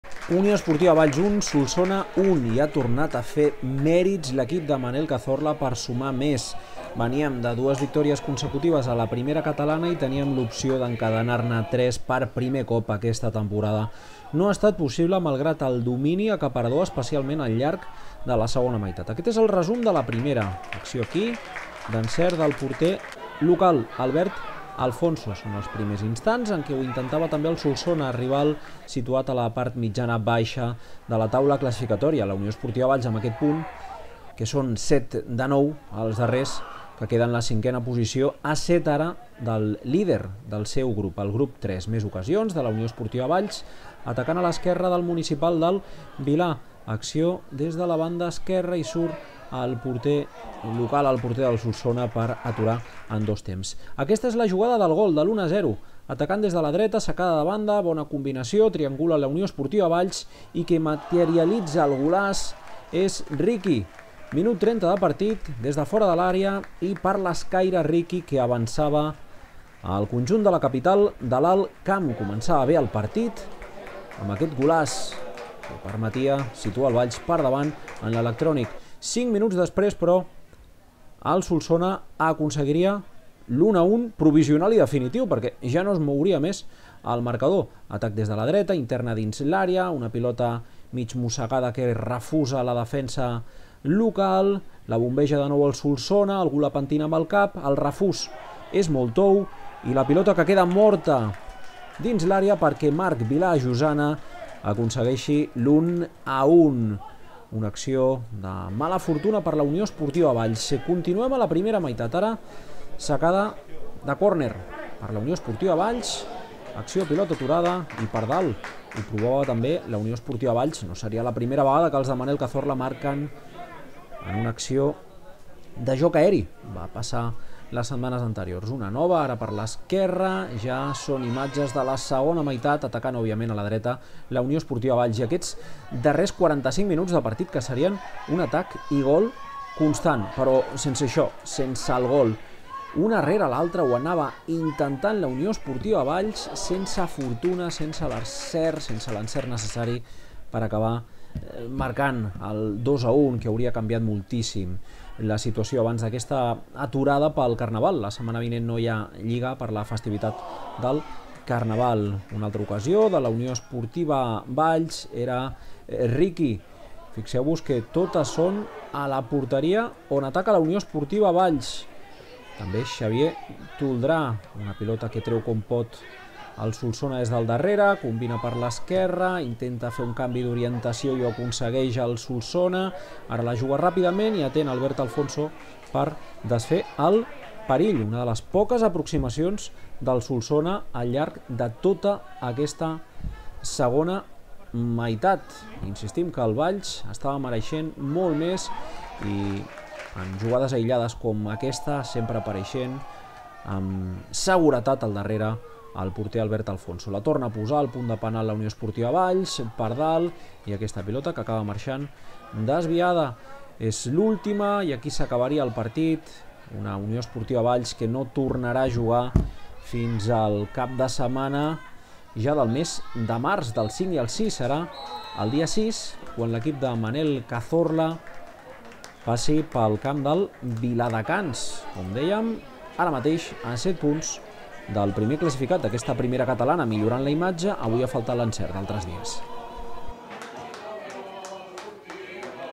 UE Valls - Solsona (1-1)
La UE Valls mereix més, però s'ha de conformar amb l'empat davant un Solsona tancat durant bona part de la segona meitat. S'escapa així l'opció d'encadenar tres victòries a la Primera Catalana.